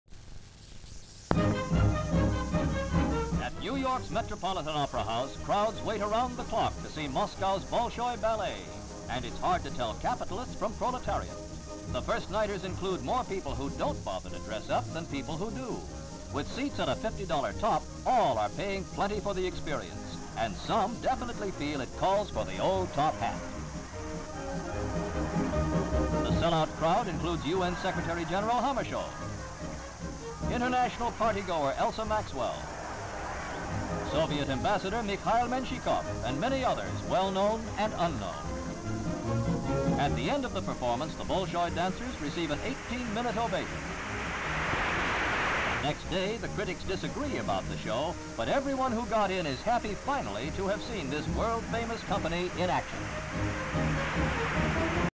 Bolshoi Ballet In New York (1950-1959)
Bolshoi Ballet in New York, United States of America (USA).

LS Metropolitan Opera House.  Various shots people waiting in large queue outside Opera House, some seated on ground, some lying, some eats picnics and drinks tea.  LS opera house, people walking past, mounted policeman in foreground.  MS queue.    MS couple in evening dress walking into Opera House.  MS UN Secretary Dag Hammerskjold entering opera house.  Various shots other personalities arriving, including American journalist Elsa Maxwell.  LS stage curtains raising, showing Bolshoi ballerinas and dancers receiving standing ovation from audience.
 FILM ID:2716.2